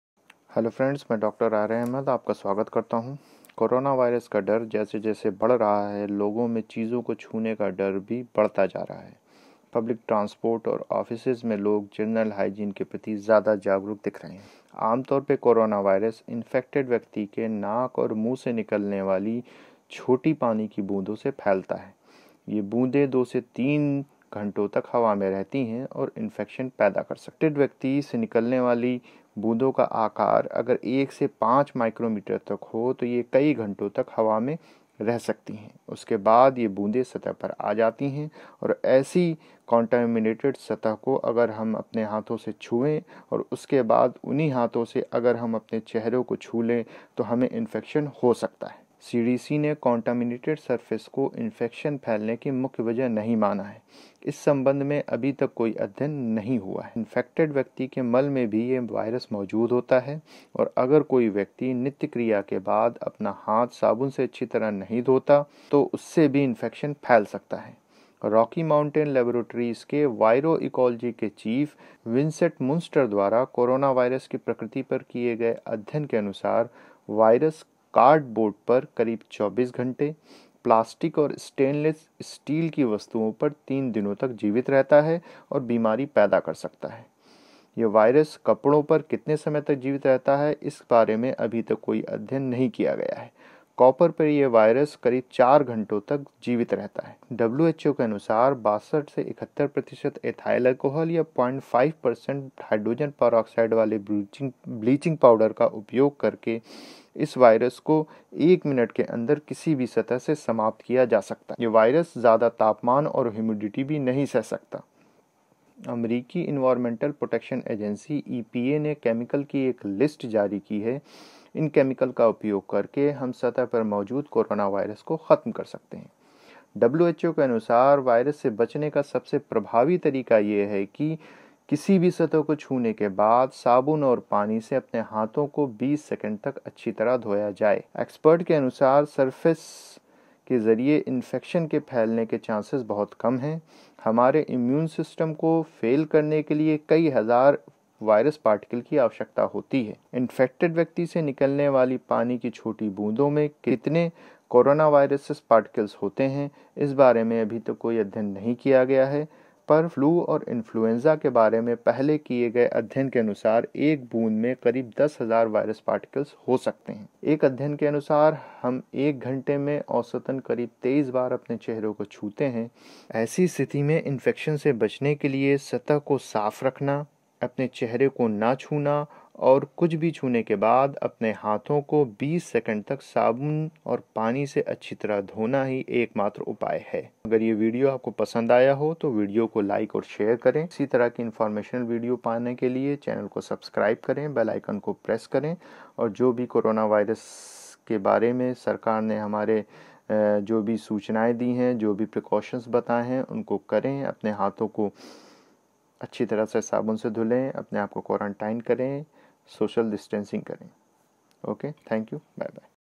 CORONAVIRUS OUTBREAK/GLOBAL PANDEMIC/CORONAVIRUS NEWS/KIS CHEEZ PER KITNI DER RAHATA HAI CORONA
DR R.A.AHMAD (HIJAMA EXPERT -  MBCS - U.K ) AT RELIEF CLINIC AND HIJAMA CENTER LUCKNOW .